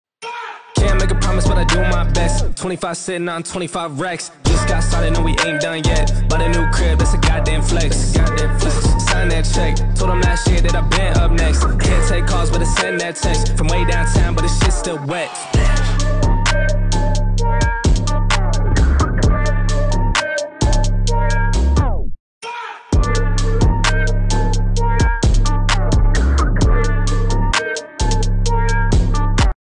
God’s True Cashmere x Nick Fouquet collaboration celebration at Just One Eye.
A night of dance, fashion, and celebrity with God's True Cashmere, Nick Fouquet and Just One Eye.